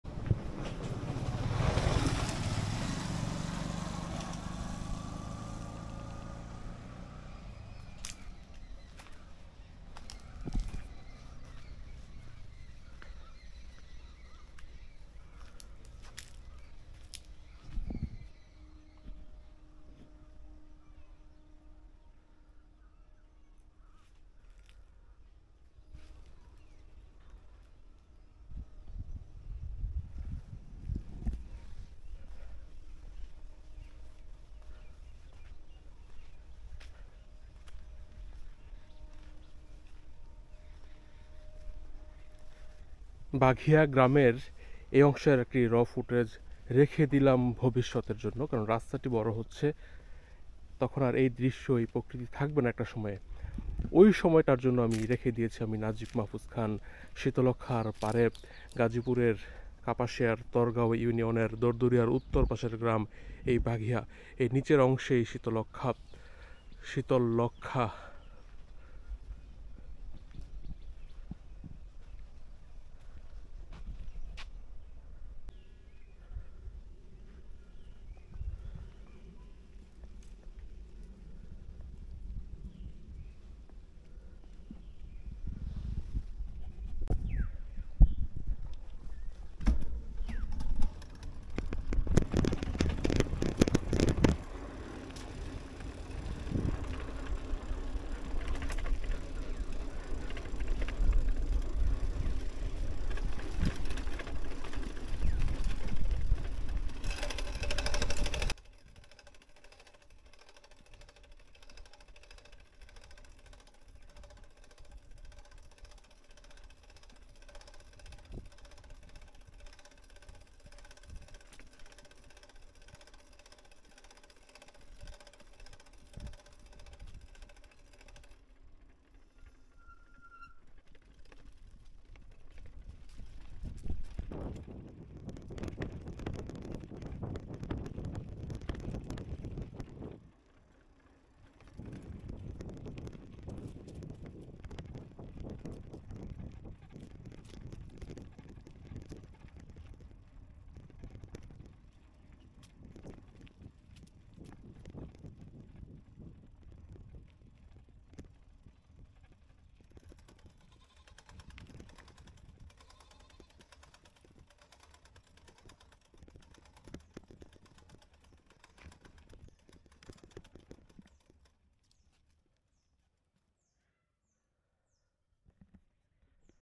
বাঘিয়া গ্রামের এই দৃশ্যটা স্মৃতি হিসেবে রেখে দিলাম ভবিষ্যৎ প্রজন্মকে দেখানোর জন্য।
বাঘিয়া গ্রামের এই দৃশ্যটা স্মৃতি হিসেবে রেখে দিলাম ভবিষ্যৎ প্রজন্মকে দেখানোর জন্য। গাজীপুরের কাপাসিয়া উপজেলার তরগাঁও ইউনিয়নের বাঘিয়া গ্রাম।নদীর পাড় ধরে দরদরিয়া যাওয়ার রাস্তা।